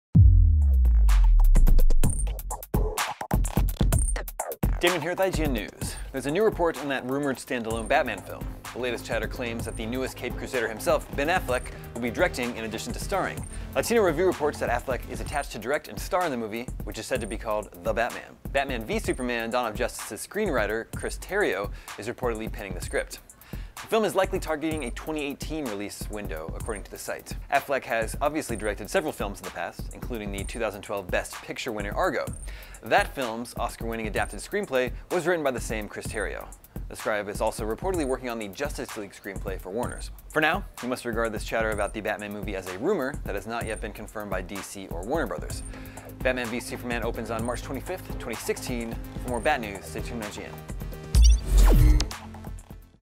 New Solo Batman Film Rumored - IGN News
There's a new report on that rumored standalone Batman film, with the latest chatter on it claiming that the newest Caped Crusader himself, Ben Affleck, will be directing in addition to starring.